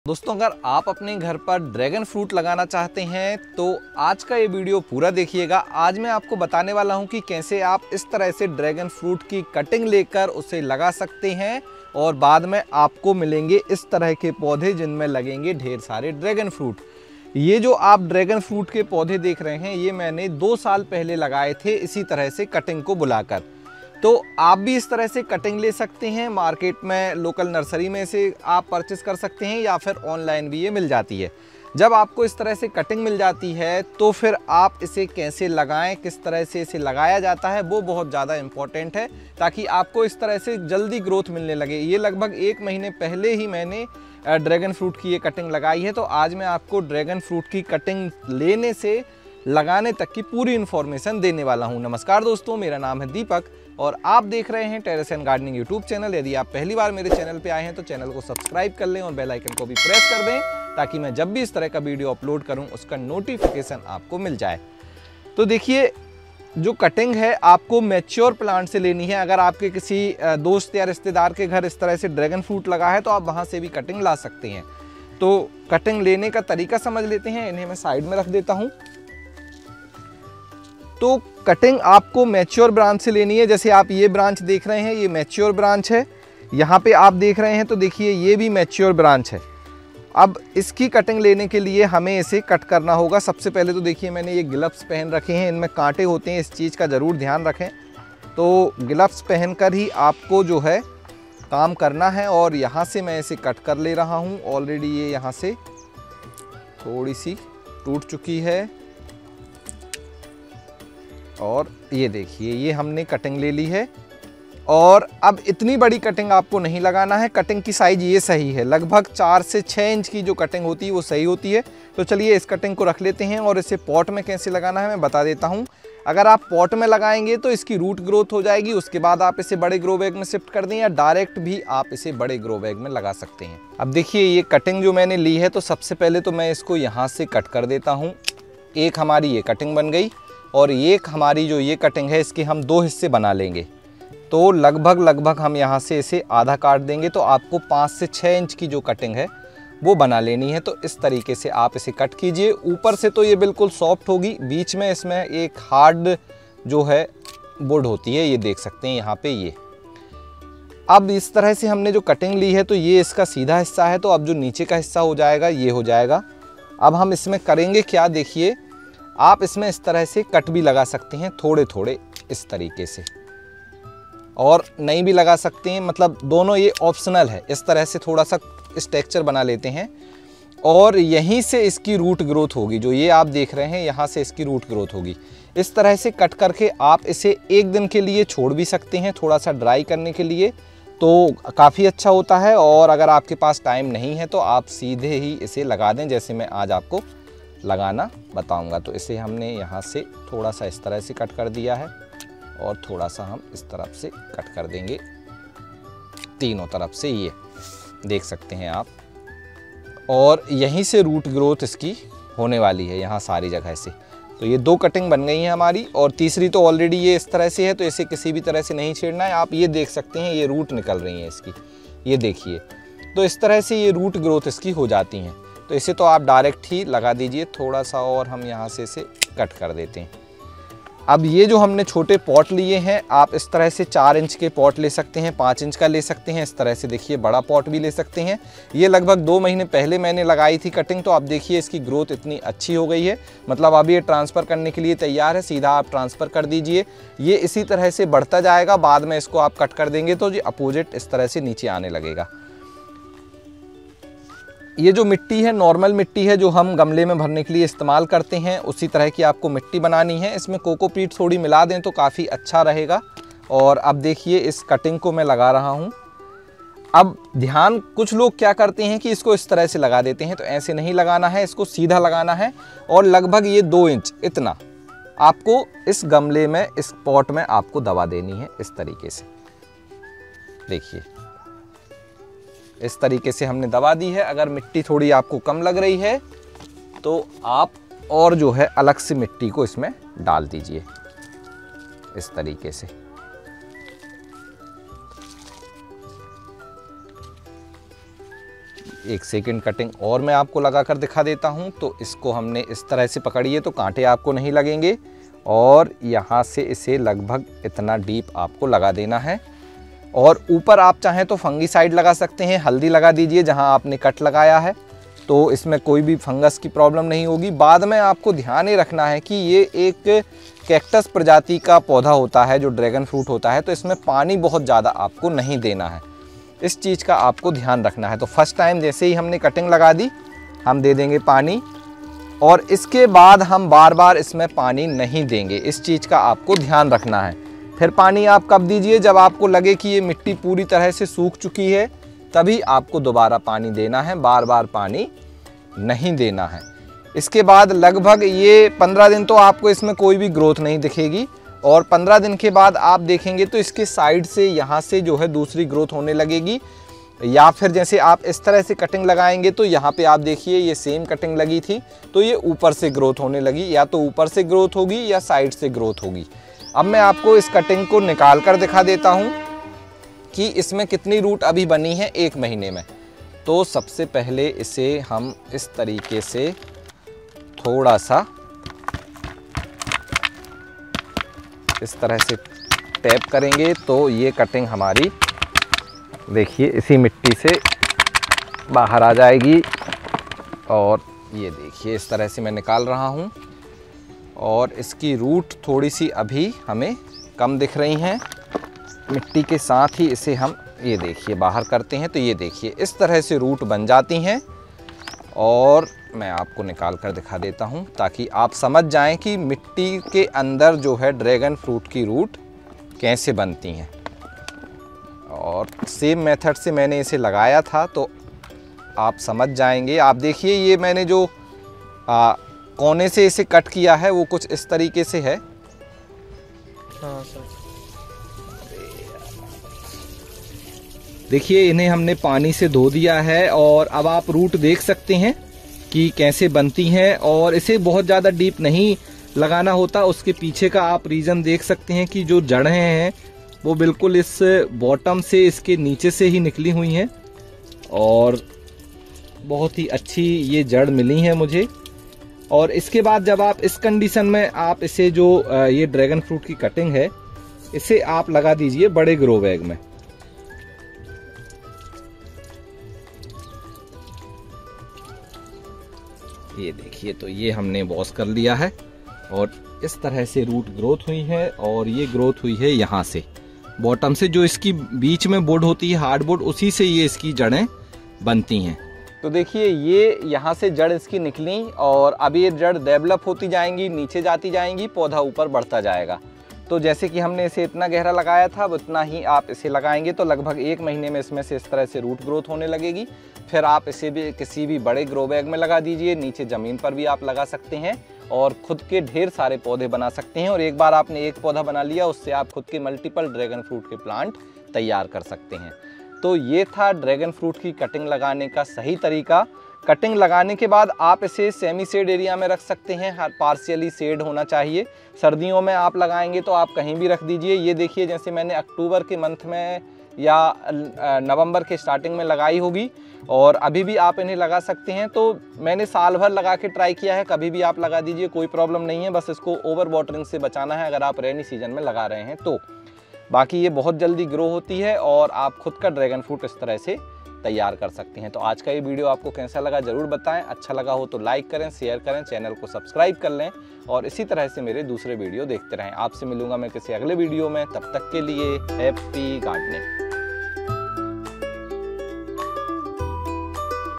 ड्रैगन फ्रूट को कटिंग से कैसे लगाये? गमले में ड्रैगन फ्रूट उगाने का आसान तरीका Dragon Fruit cutting
गमले में ड्रैगन फ्रूट उगाने का आसान तरीका जानें ड्रैगन फ्रूट को कटिंग से कैसे लगाये? How to plant dragon fruit from cutting in Hindi
Like Terrace & Gardening Facebook Page for More Updates
FB.me/Terraceandgardening